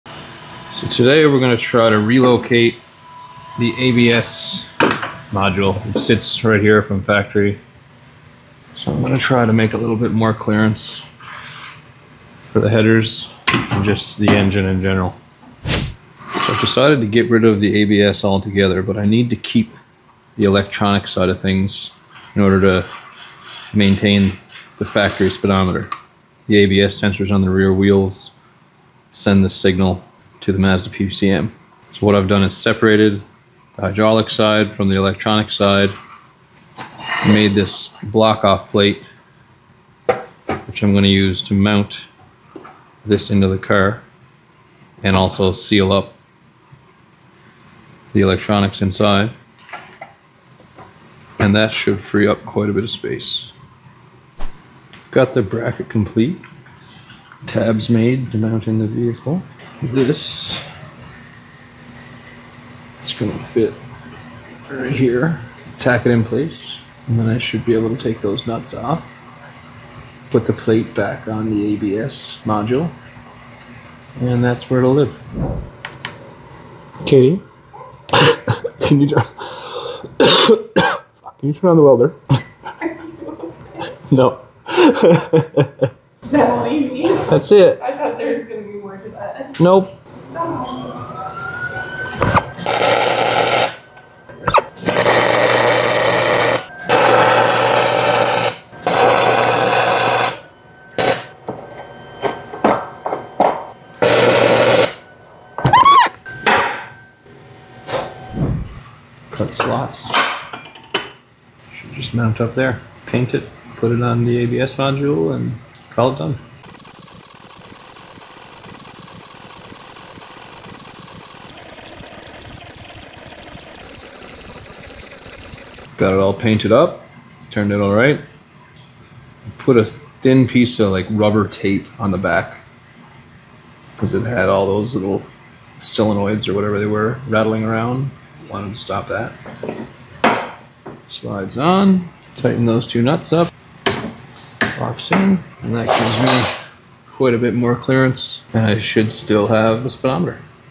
Mazda RX8 LS Swap ABS Delete/Relocation
Deleting the ABS in my mazda rx8 in an attempt to make space for the LS engine. Removing the ABS but maintaining the electronics, should allow the PCM to receive a speed signal from the sensors on the rear wheels and maintain the functionality of the factory speedometer.